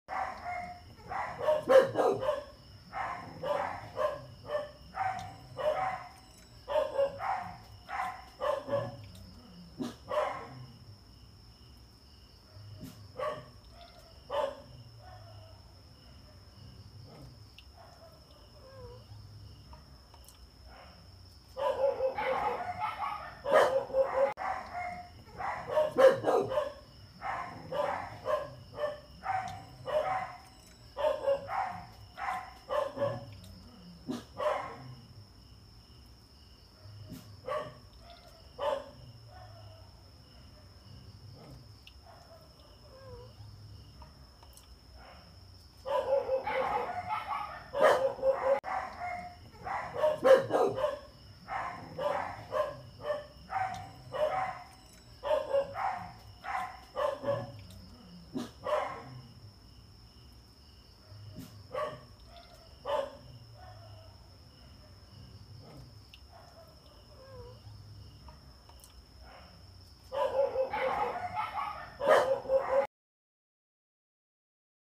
House centipede encounter for the first time
talimbabaga is in the house. Daw kahaladlokan gid iya appearance 🧐🤔🙀